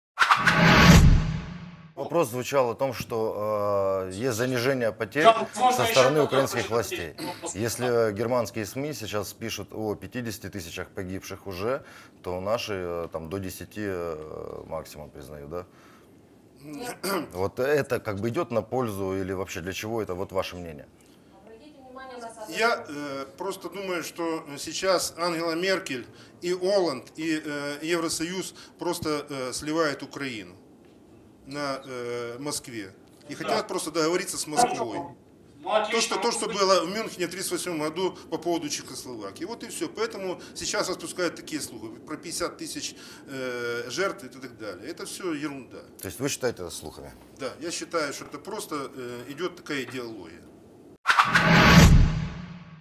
ЕС "сливает" Украину России - отсюда слухи об огромном числе погибших на Донбассе
Информация о чрезвычайно большом количестве погибших в конфликте на Донбассе, появившаяся в последнее время в западных СМИ, связана с намерением Евросоюза отдать Украину на откуп России. Такое мнение высказал в ходе телемоста "Киев - Донбасс" в эфире "17 канала" представитель Киевского союза учасников боевых действий в АТО Сергей Гринчук.

Смотрите также:

Телемост "Киев-Донецк: Ищем общий язык". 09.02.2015